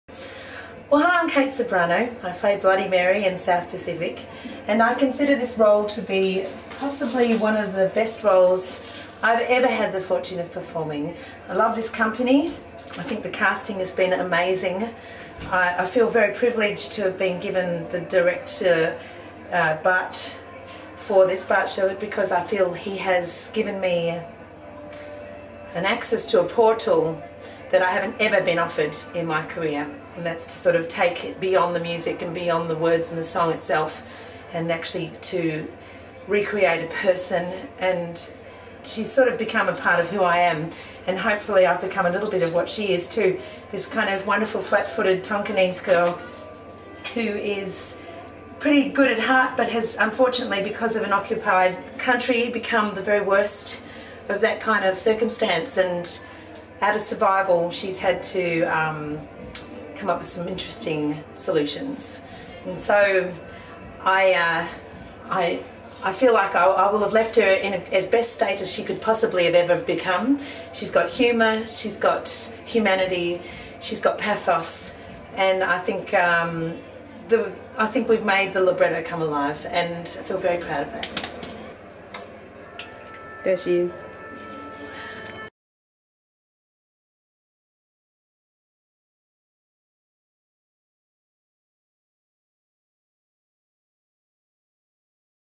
A quick chat with Kate Ceberano
After 7 Tony Awards and sell out seasons on Broadway and the West End, the Lincoln Center Theatre's stunning production of Rodgers & Hammerstein's SOUTH PACIFIC is finally coming to Sydney, Melbourne and Brisbane.